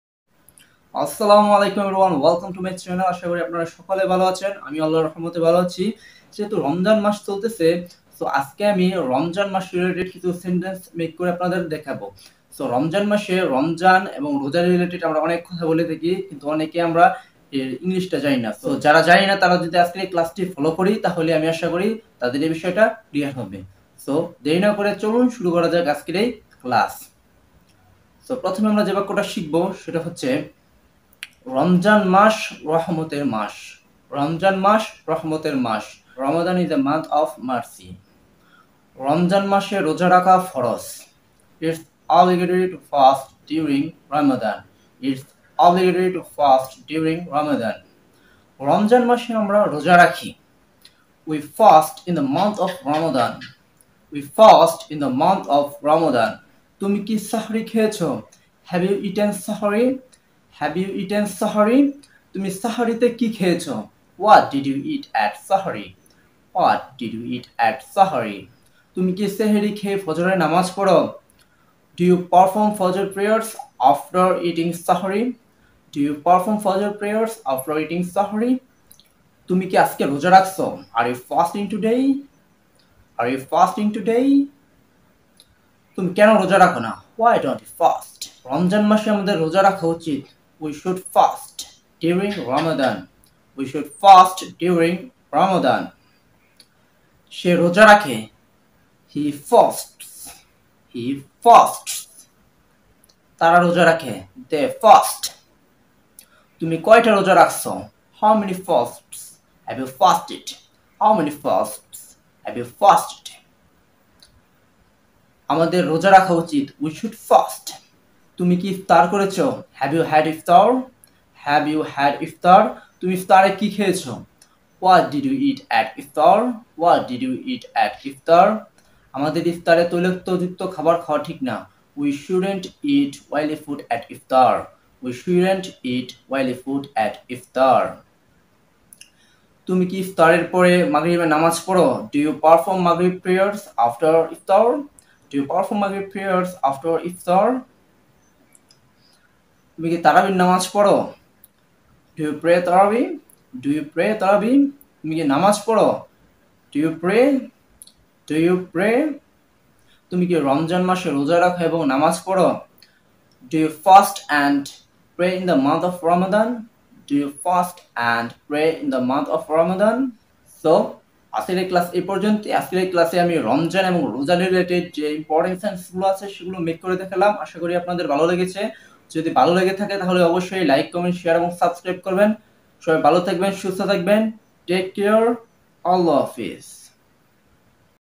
রমজান এবং রোজা related কিছু গুরুত্বপূর্ণ ইংরেজি বাক্য ।
Welcome To Learn English With Rafiour.

In today's class I have discussed some sentences about Ramadan and fasting. This class is really helpful for spoken english.

So watch the video and learn these sentences.